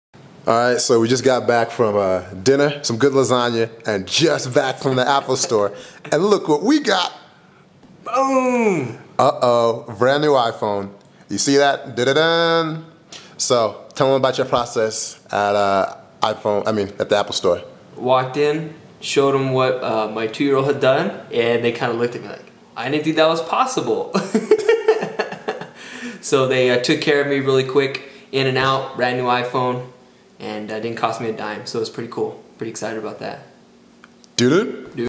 IPhone 4 Is Back Alive from Apple Peeps are COOL PEOPLE
Just got back from Apple store and they hooked us up..